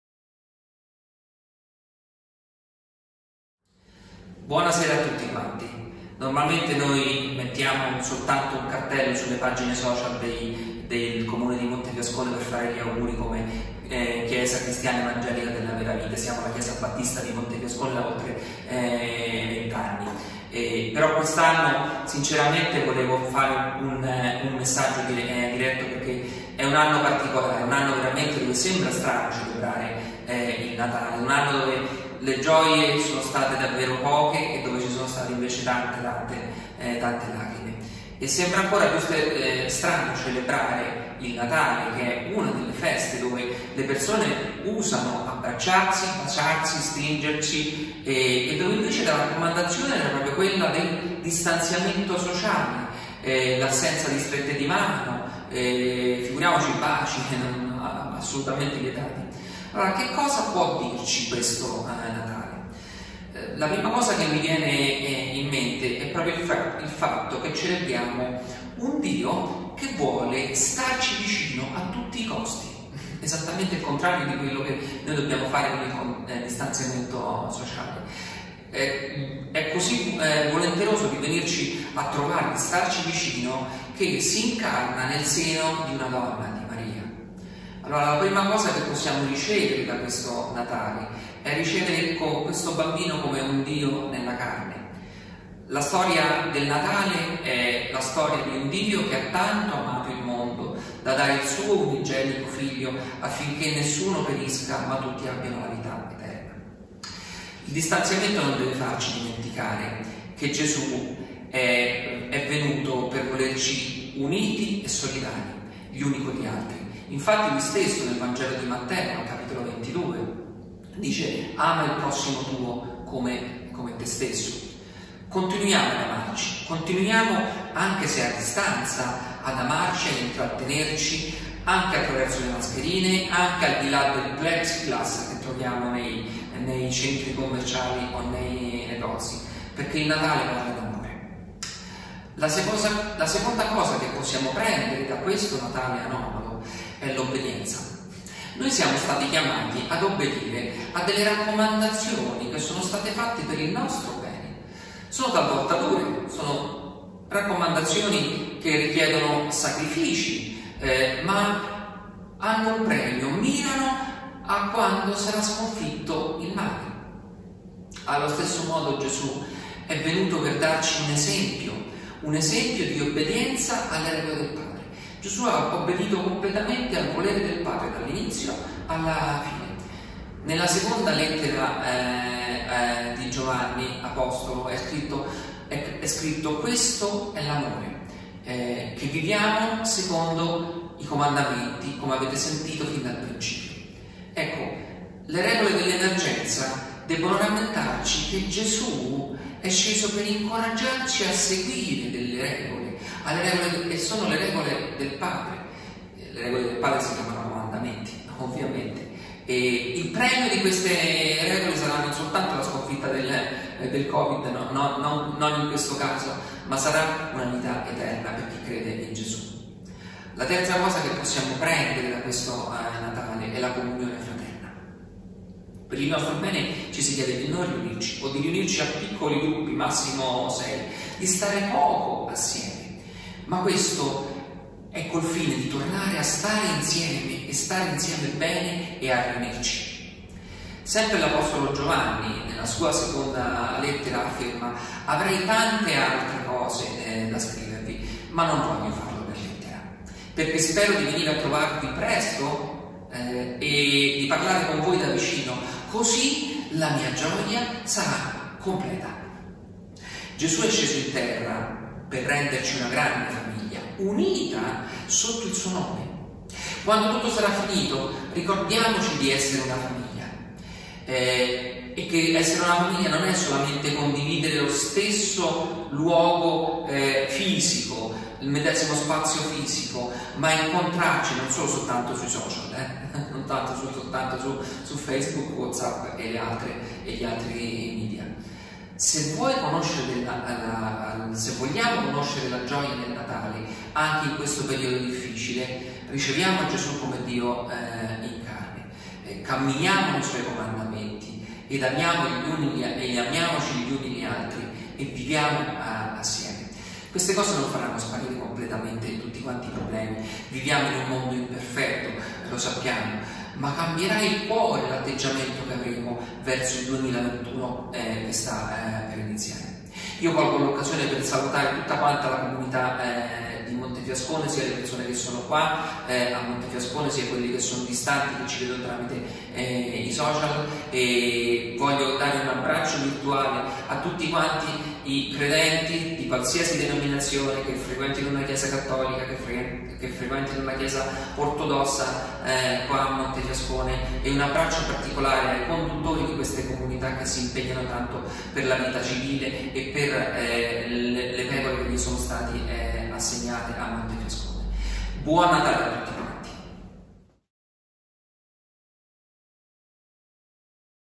Natale 2020
Cosa possiamo salvare di questo Natale "anomalo" in tempo di Covid?
Brani utilizzati:

"Dio ha tanto amato il mondo da dare il suo unico Figlio, affinché chiunque crede in lui non perisca, ma abbia vita eterna."(Giovanni 3:16)

"Ama il tuo prossimo come te stesso".  (Matteo 22:39)

“Questo è l'amore: che viviamo secondo i suoi comandamenti e come avete sentito fin da principio.”. (2 Giovanni 6)

“Avrei ancora tante cose da scrivervi, ma non voglio farlo per lettera, perché spero di venire a trovarvi presto e di parlarne con voi da vicino. Così la nostra gioia sarà completa.” (2 Giovanni 12)